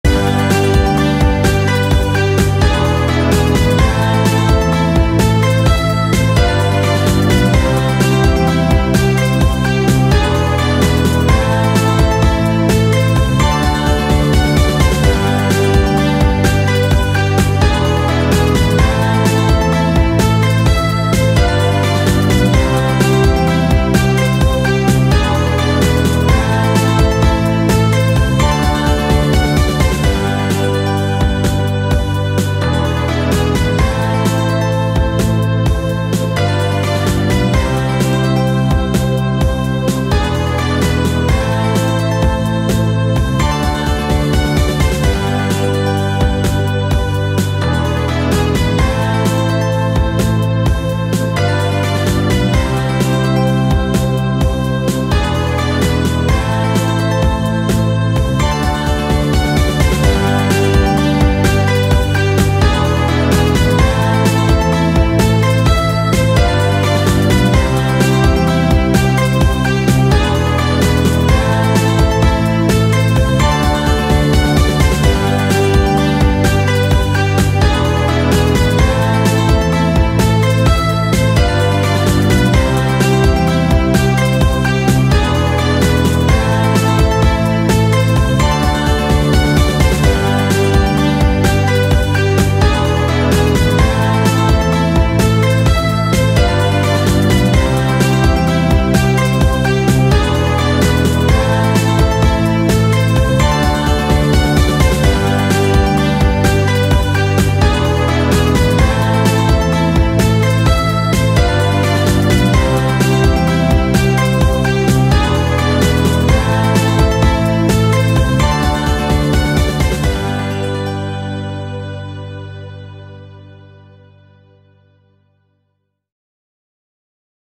Title theme for a video game
Title theme.
My friends were making a video game and tasked me with doing the music. I only got around to finishing a few of them, however. This is one of them.